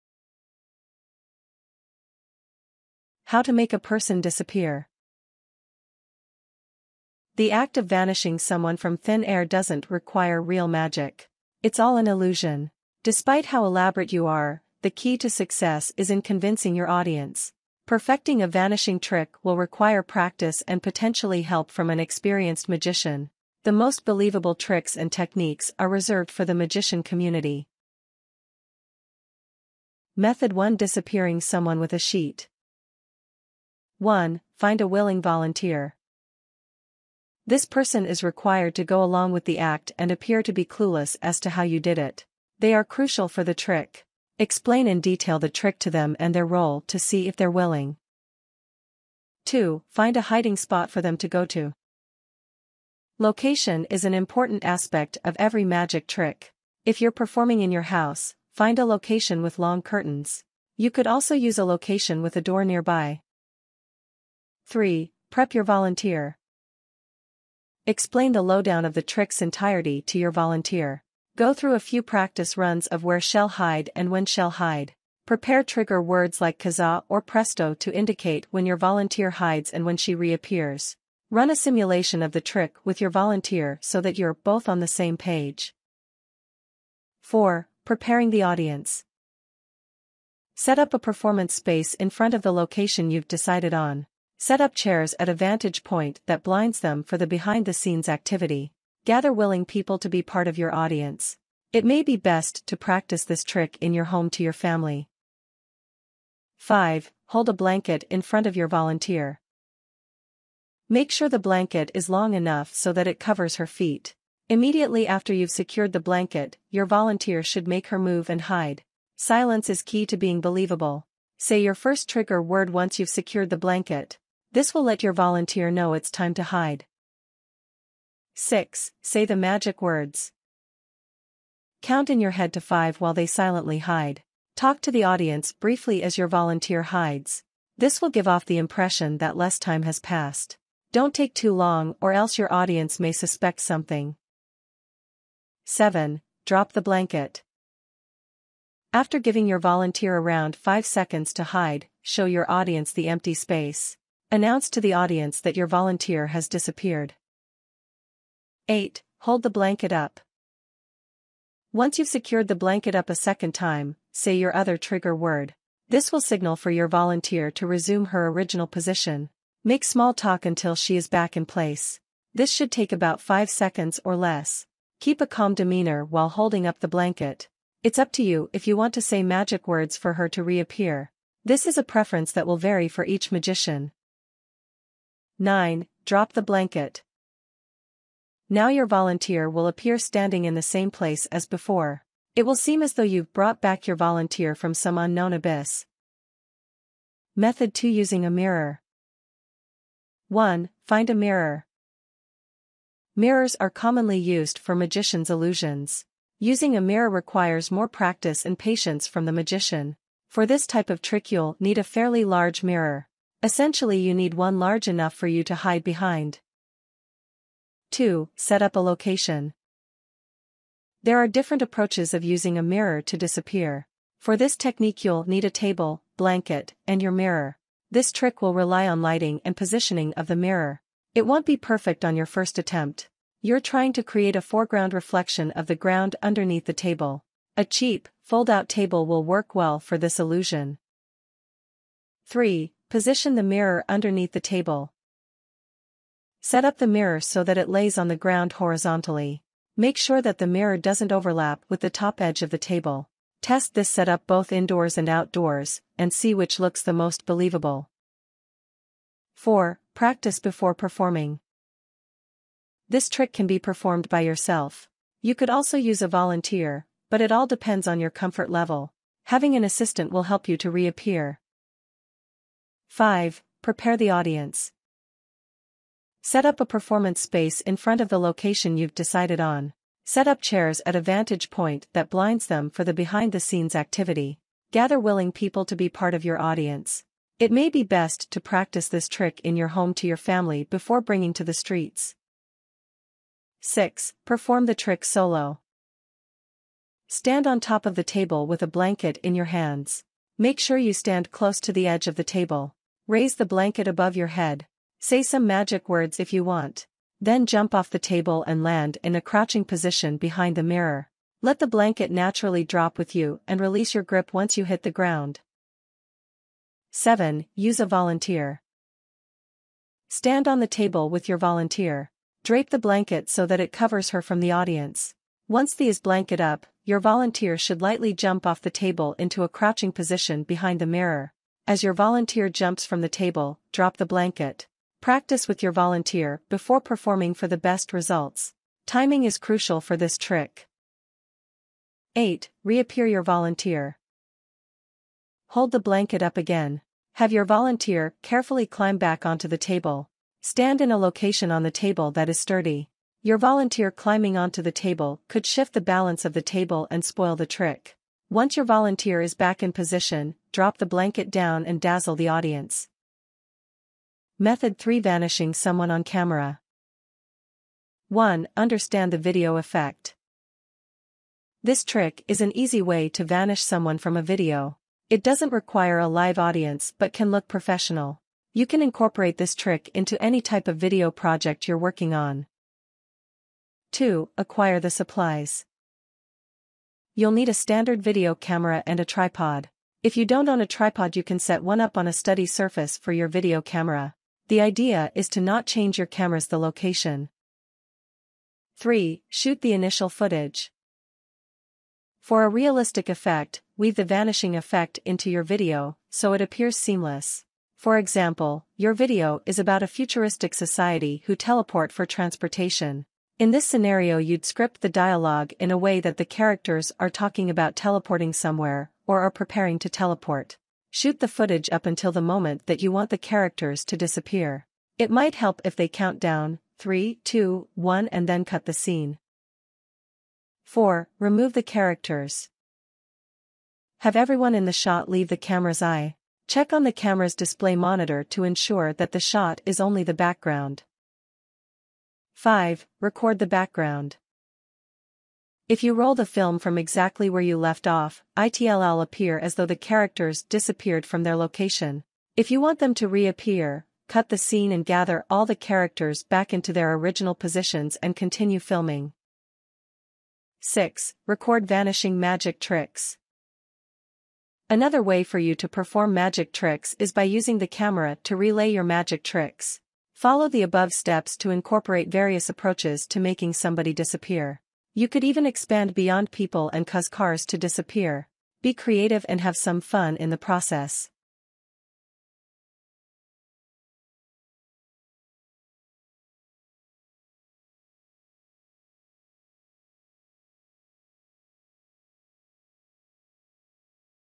Master the Art of Vanishing: 3 Mesmerizing Methods Revealed! Sheet, Mirror, and Camera Magic
In this video description, the content revolves around three methods of making things disappear, specifically focusing on making a person vanish. The first method involves using a sheet, where a volunteer hides behind a blanket, and with well-timed trigger words and audience misdirection, it appears as though the person has disappeared and reappeared. The second method utilizes a mirror, creating the illusion of disappearance by hiding behind the mirror strategically positioned under a table. The third method explores how to achieve the disappearance effect on camera, emphasizing the use of video editing techniques to make someone vanish seamlessly from a scene. The description also encourages creativity and suggests expanding the tricks to include various objects or even cars. Additionally, the use of trigger words and precise timing is highlighted throughout the methods.